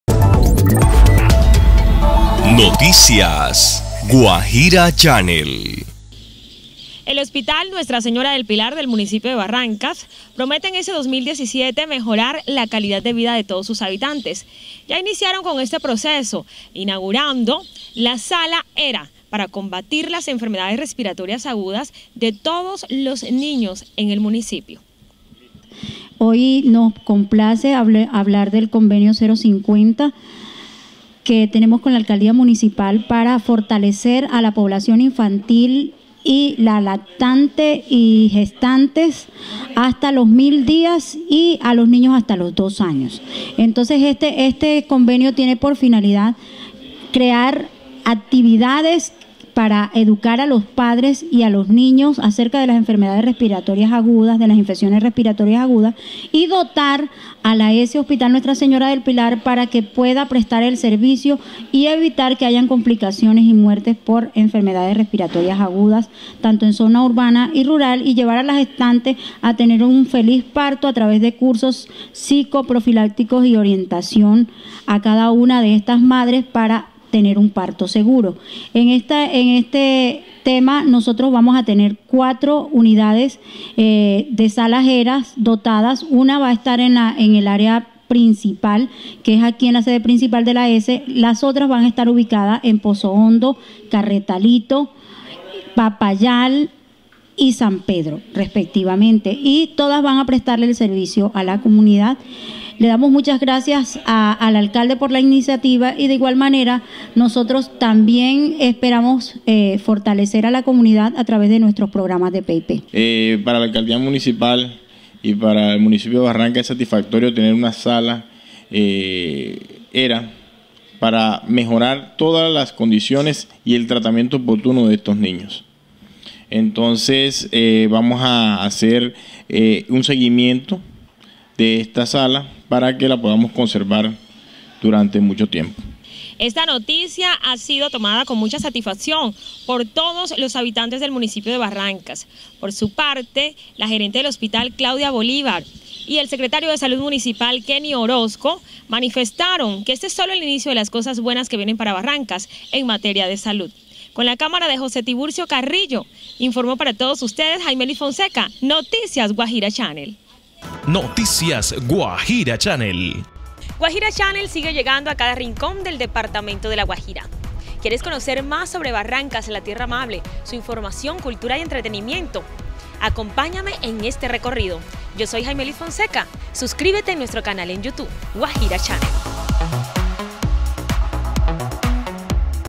INAUGURAN SALA ERA EN HOSPITAL DE BARRANCAS
La inauguración  de esta sala permitirá mejorar la calidad de vida de niños en barrancas.